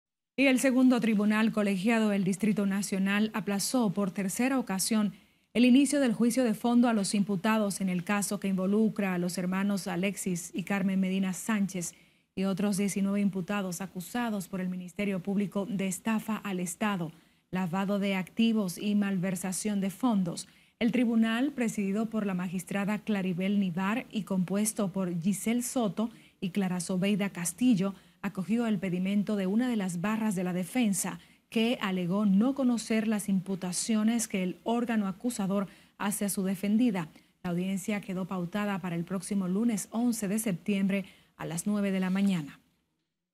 Aplazan para el 11 de septiembre inicio de juicio fondo caso Antipulpo
El Segundo Tribunal Colegiado del Distrito Nacional aplazó por tercera ocasión el inicio del juicio de fondo a los imputados en el caso que involucra a los hermanos Alexis y Carmen Medina Sánchez y otros 19 imputados acusados por el Ministerio Público de estafa al Estado, lavado de activos y malversación de fondos.

La audiencia quedó pautada para el próximo lunes 11 de septiembre, a las 9:00 de la mañana.